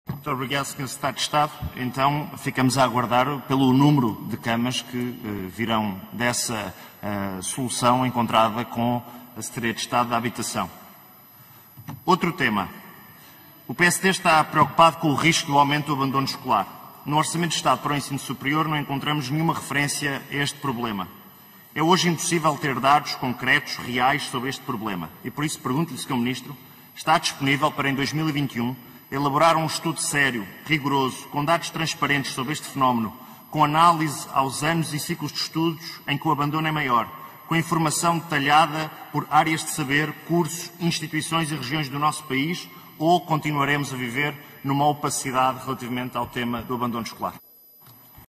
Alexandre Poço questiona o Ministro da Ciência, Tecnologia e Ensino Superior
29 de outubro de 2020